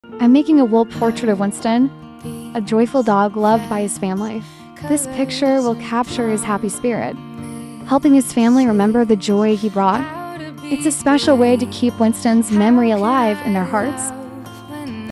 I Bring Winston Home How? Custom Wool Pet Portraits
Celebrate your furry friend with a handcrafted custom wool felt pet portrait! This unique and personalized artwork is a perfect way to honor your pet and keep their memory close to your heart. Check out our needle felting process and see how we turn your pet photos into a one-of-a-kind masterpiece. Perfect for pet owners or as a gift for a loved one.